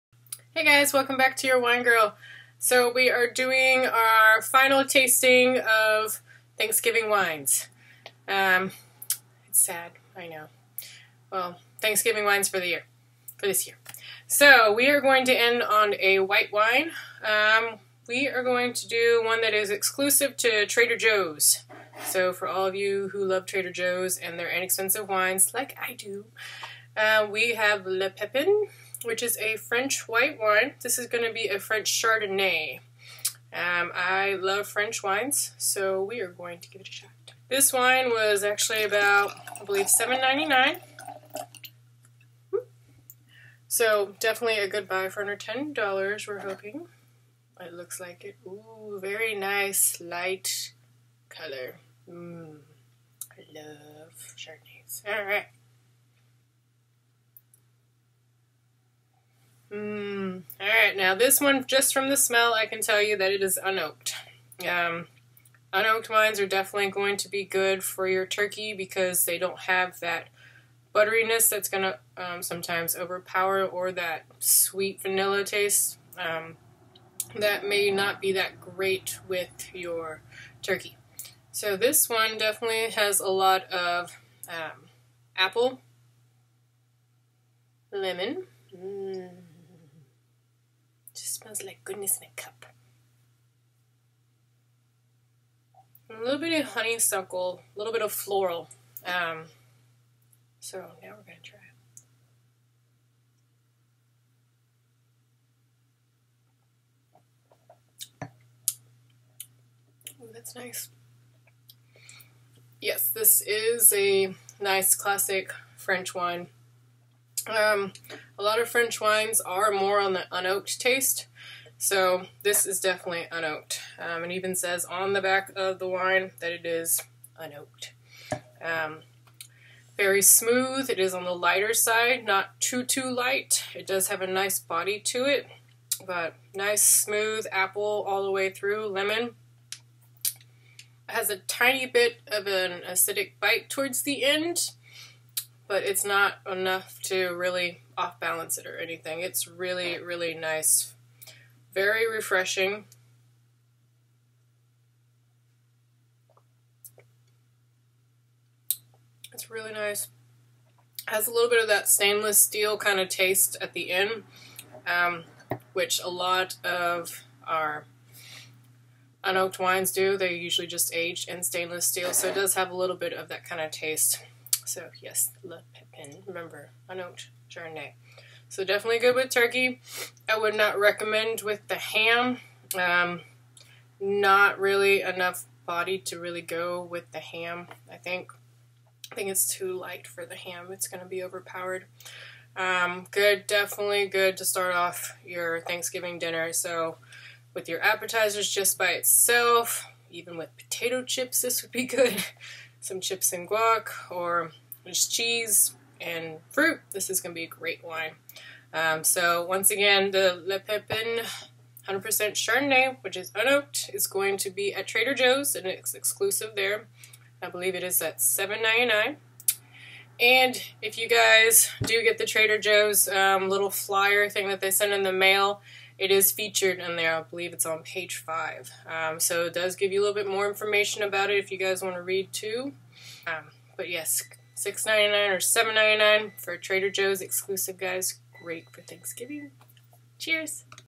Thanksgiving Dinner "Wine With Turkey" ep #4 Le Pépin Chardonnay 2014
White Wine with your THANKSGIVING dinner... TURKEY WINE pairing! This is Episode #4 of the Fall pairing for your Thanksgiving Day dinner. and ep #2 and episode #3

WINE FOR TURKEY
WHIE WINE
Le Pépin Chardonnay 2014

Find this wine at Trader Joes

WINE TO SERVE WITH TURKEY

your wine girl